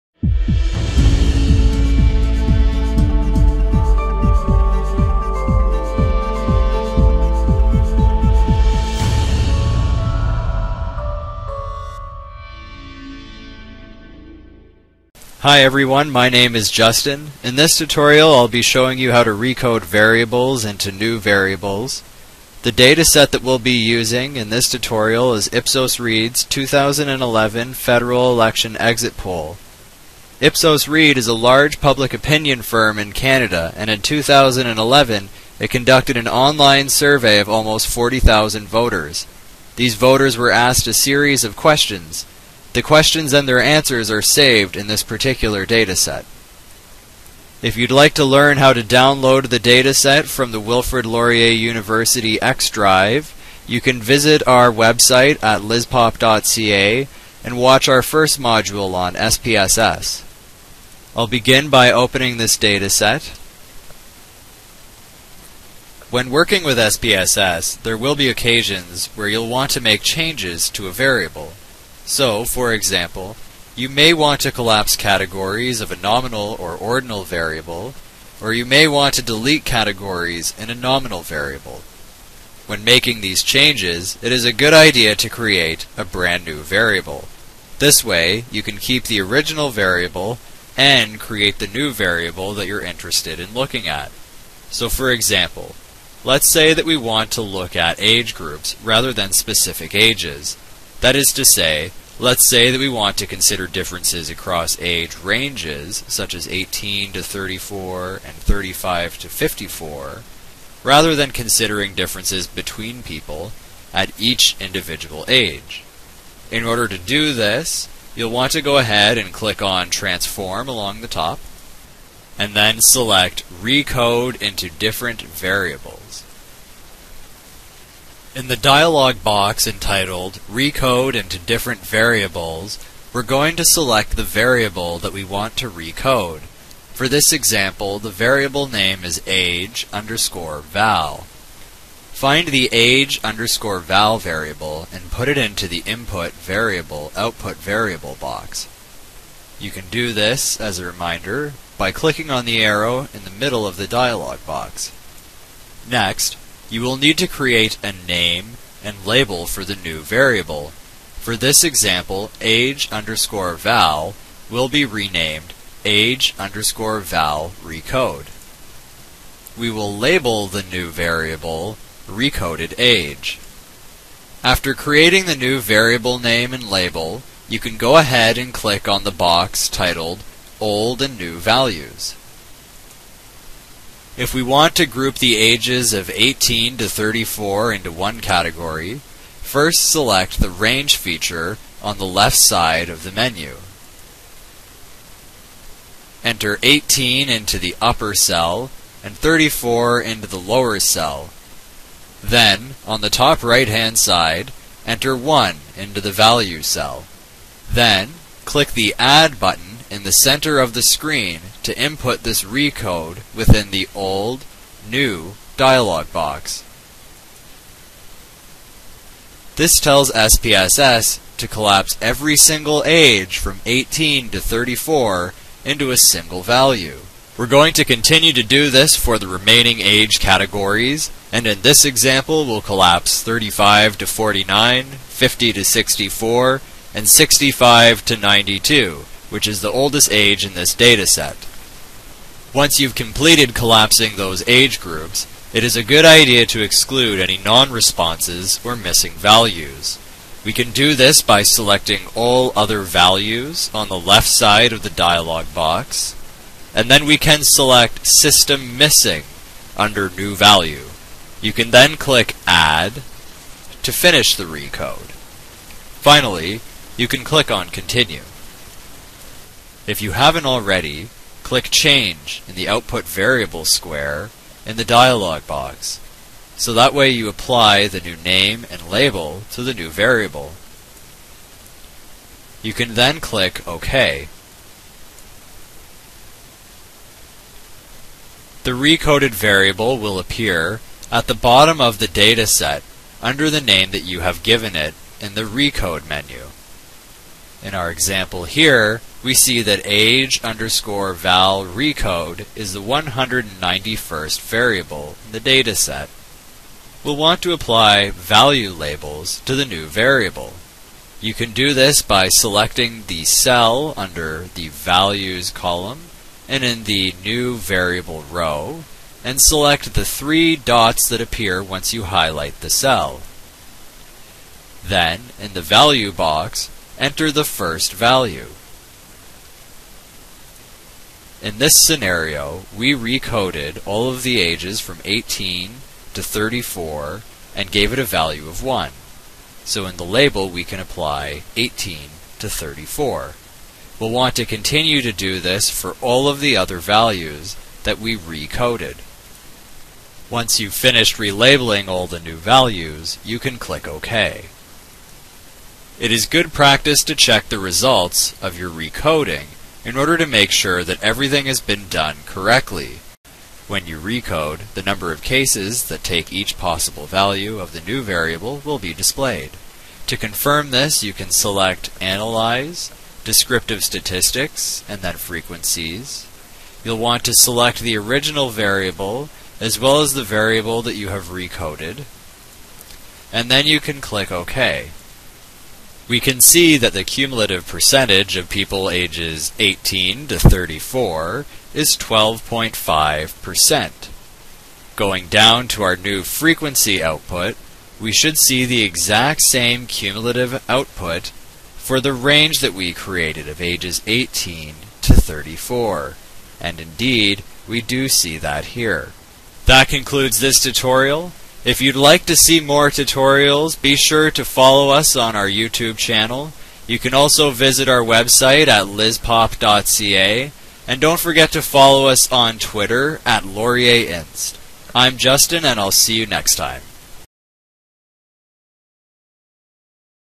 LISPOP SPSS Module #2 - Recode Variables
In this tutorial, we show you how to create new variables and recode variables using SPSS.